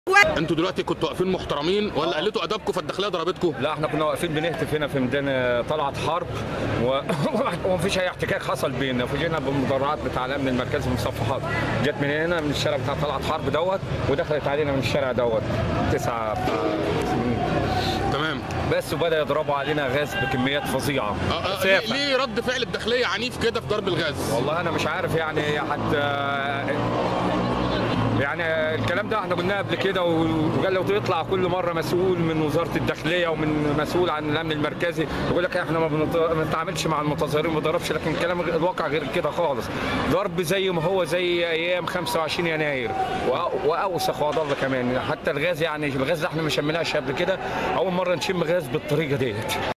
الفيديو الوحيد للشهيد محمد فؤاد المنياوى قبل إغتياله
تم تصوير الفيديو اثناء اشتباكات طلعت حرب